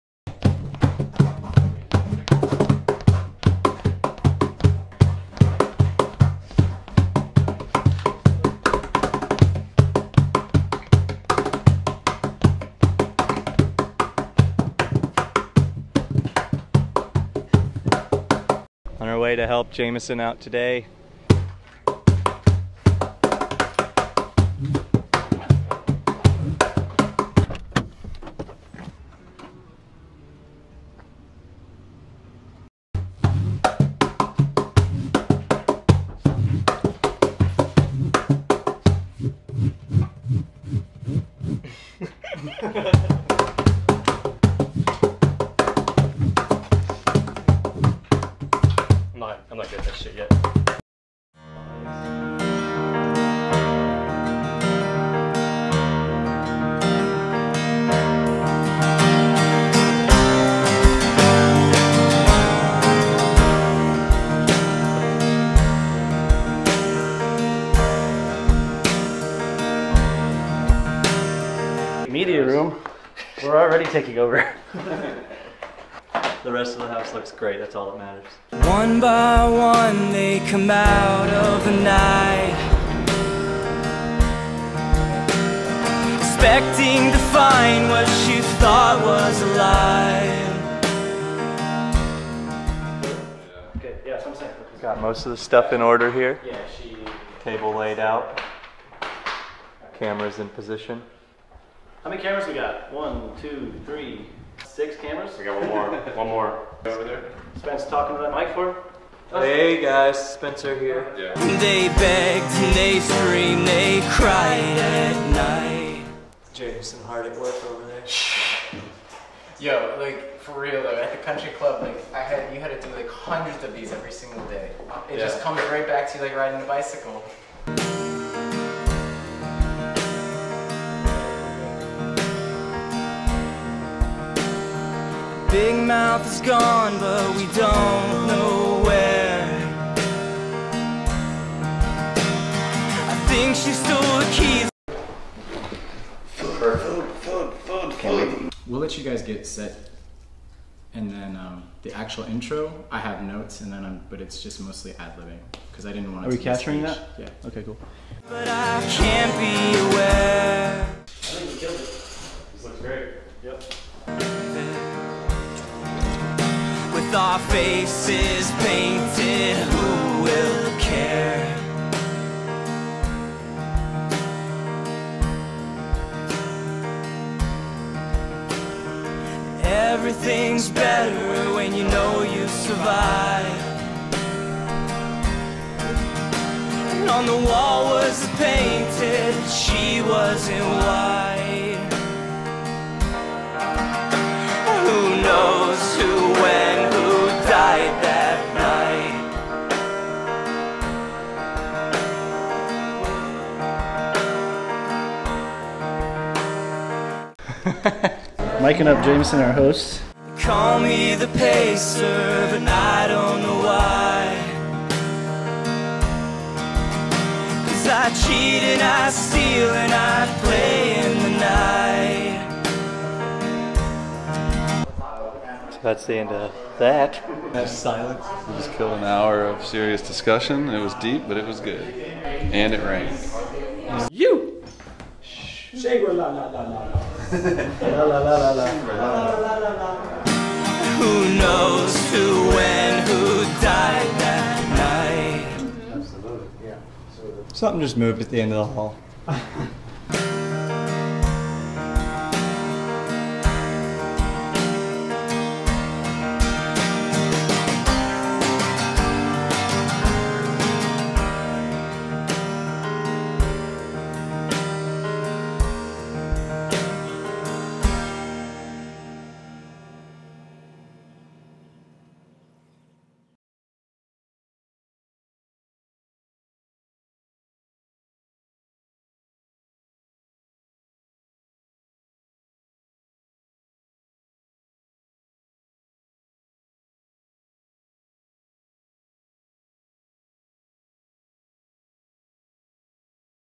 Documentary Behind the Scenes - Death over dinner
Behind the scenes look at  a Death Over Dinner discussion that took place at the Shangri La In Bonita Springs, Fl